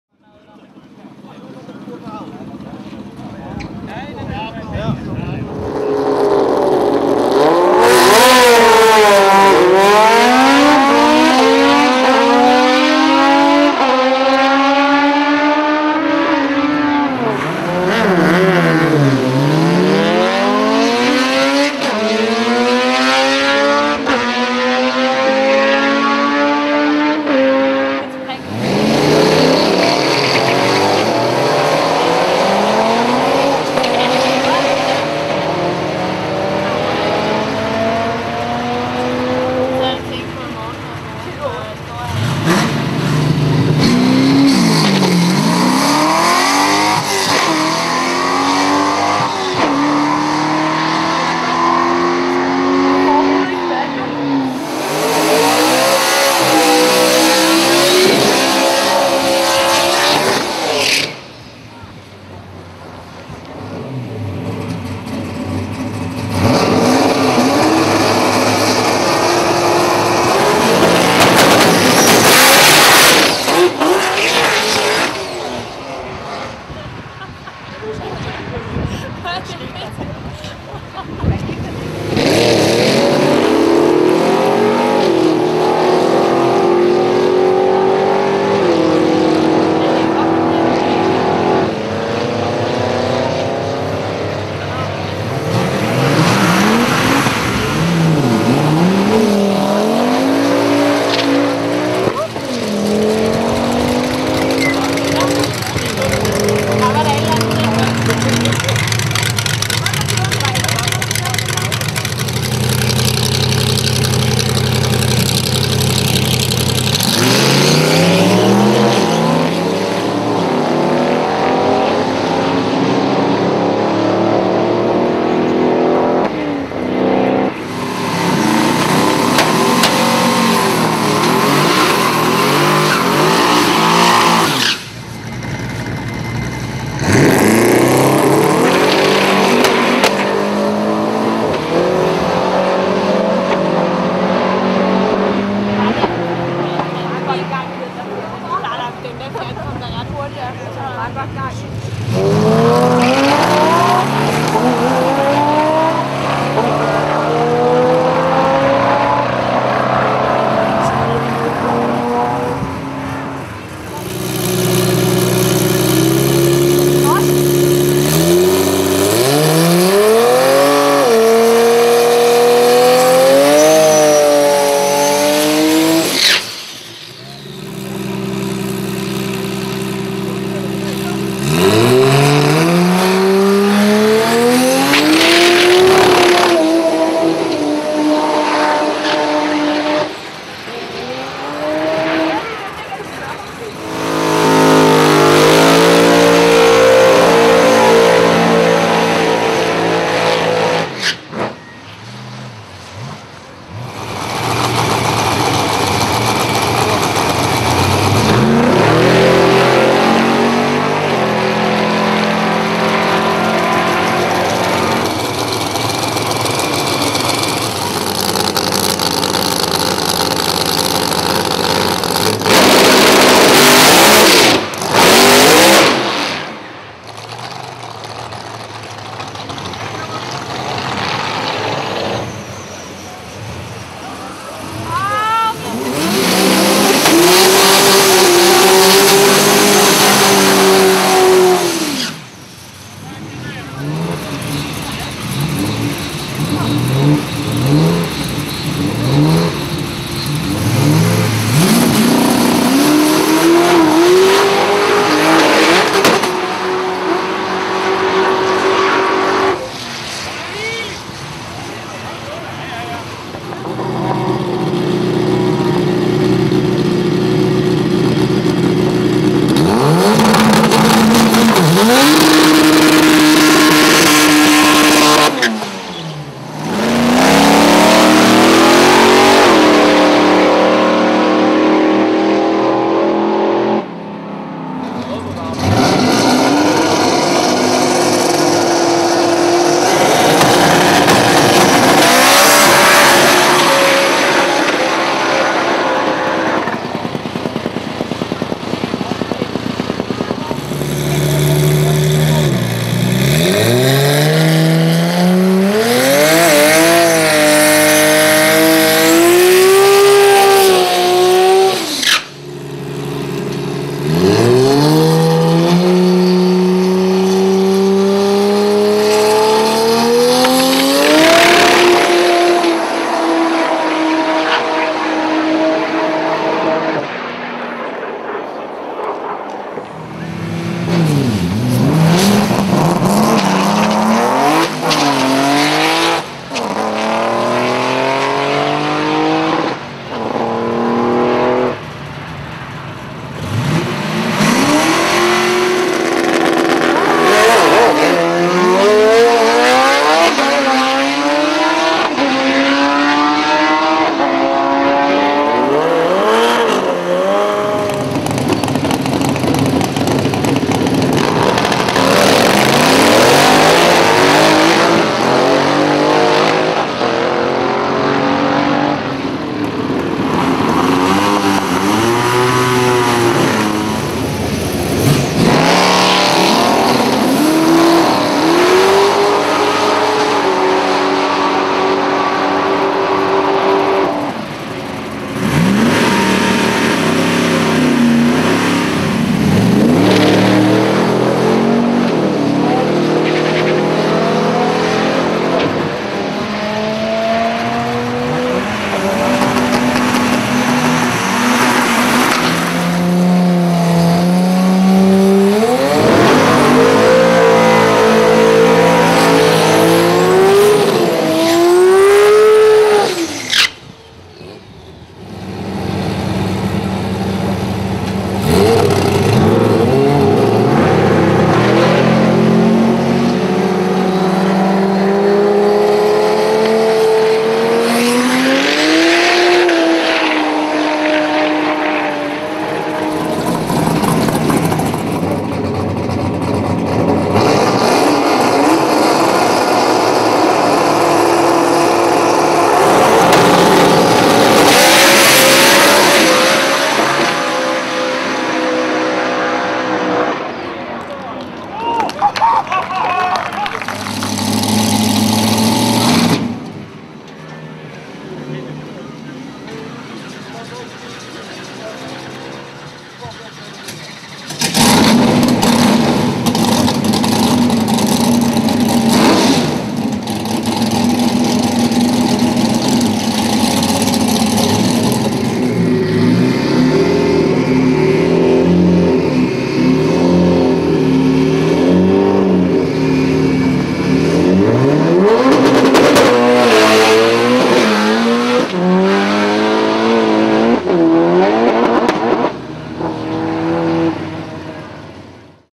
DragRace MECA RaceWay 09-09-09
Dragrace at MECA RaceWay, Malmø, Sweden.
Many Swedish and Danish Dragracers...
Filmed by EfterZ.com